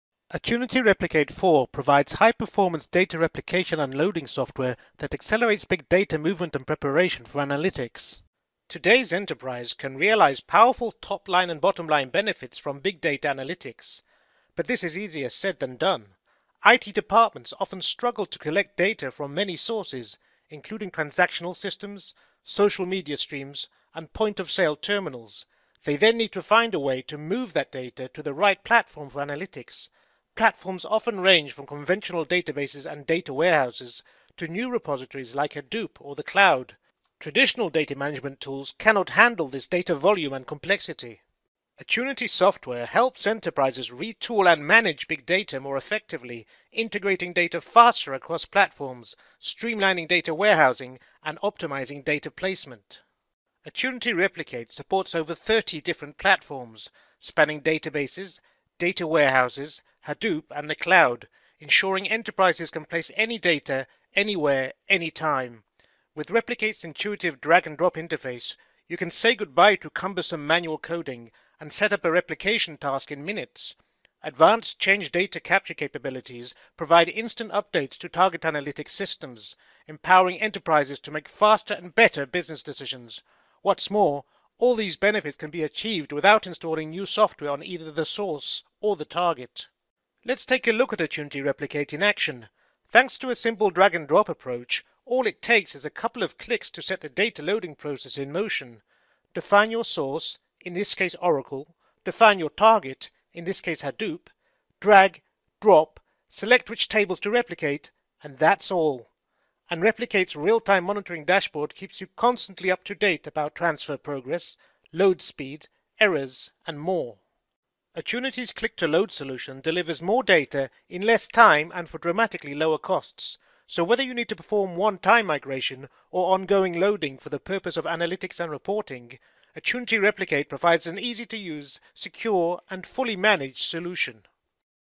Attunity Replicate 4 - a High-Performance Data Replication & Loading Solution
Moving data across an enterprise can be challenging. But it doesn’t need to be. Watch this video to see Attunity Replicate in action - as it simply, quickly and efficiently replicates Oracle data to Hadoop. This high-performance, easy-to-use data replication software, supporting a broad range of enterprise data sources and targets, enables organizations to accelerate and reduce the costs of moving any data, any time, anywhere. Built to meet a variety of mission-critical business needs, Attunity Replicate features zero footprint technology and a Click-2-Replicate design. The software, featuring TurboStream change data capture (CDC) technology, is redefining data replication by drastically simplifying the user experience and replacing the need for complex and costly custom development projects. Attunity Replicate is also fast and easy to learn and implement, providing quick time-to-value and the lowest TCO on the market. Check it out today!